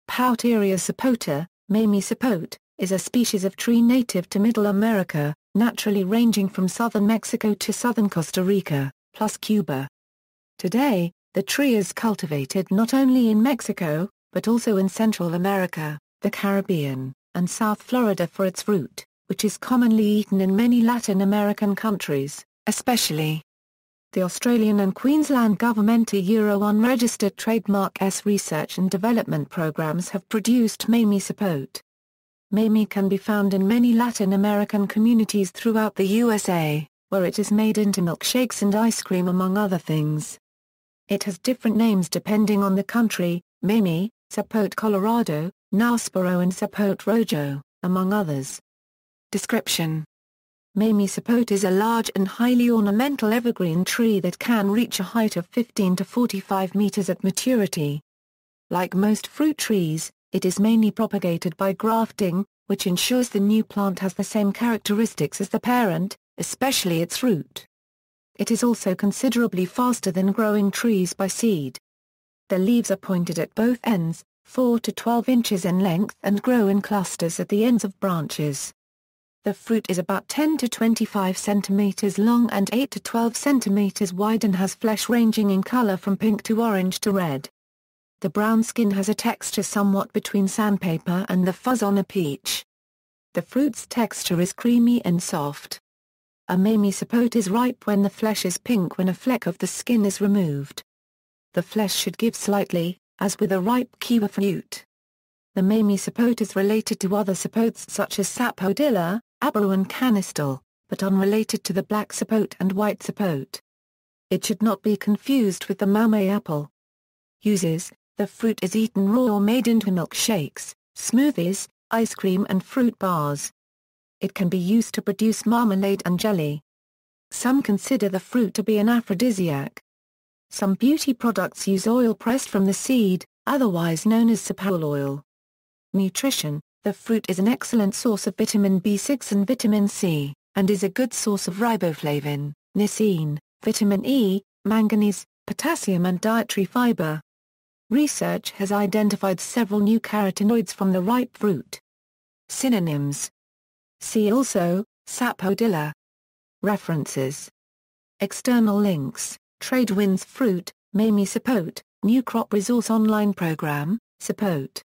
Pouteria sapota
Pouteria sapota, mamey sapote, is a species of tree native to Middle America, naturally ranging from southern Mexico to southern Costa Rica, plus Cuba. Today, the tree is cultivated not only in Mexico, but also in Central America, the Caribbean, and South Florida for its fruit, which is commonly eaten in many Latin American countries, especially. The Australian and Queensland Government’s research and development programs have produced mamey sapote. Mamey can be found in many Latin American communities throughout the USA, where it is made into milkshakes and ice cream among other things. It has different names depending on the country: mamey, zapote colorado, níspero and zapote rojo, among others.